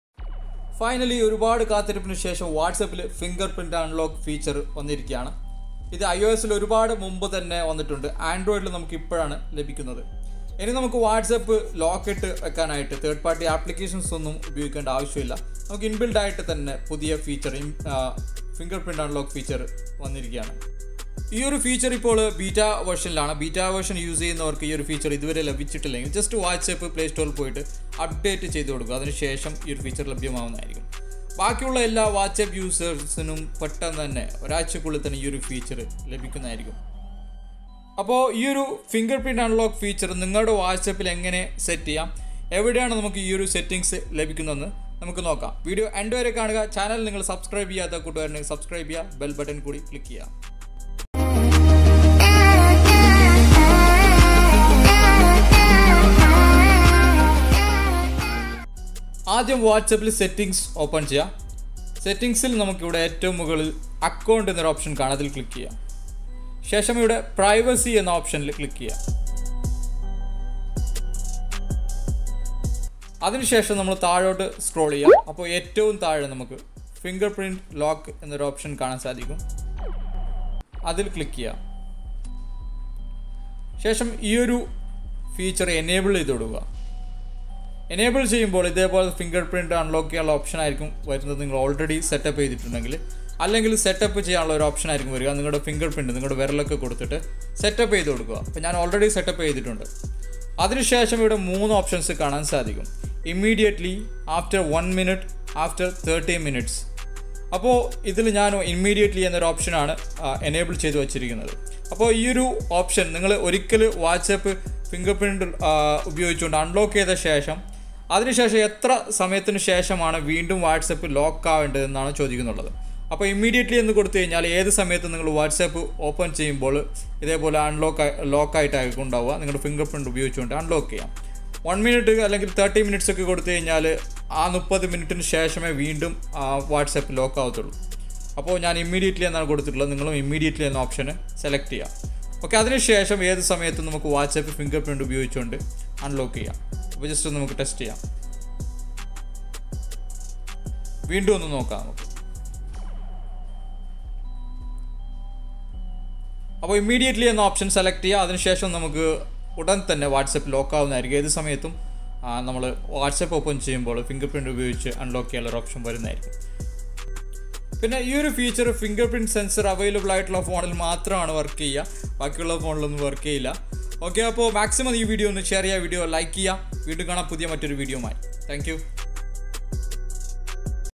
WhatsApp ൽ ഫിംഗർ പ്രിന്റ് ലോക്ക് ഫീച്ചർ | Whatsapp Fingerprint Lock Feature 2019
Whatsapp Latest Update - Fingerprint Lock Feature 2019 - Whatsapp tips and tricks malayalam